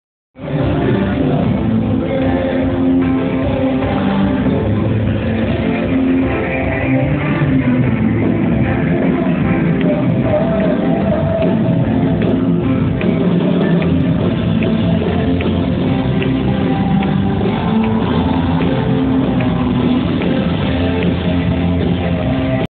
MIEL SAN MARCOS de GUATEMALA West Palm Beach en vivo. 2015.
Miel SanMarcos desde Guatemala.
Salmista Wendy Gonzalez y mas
En West Palm Beach Florida. 2015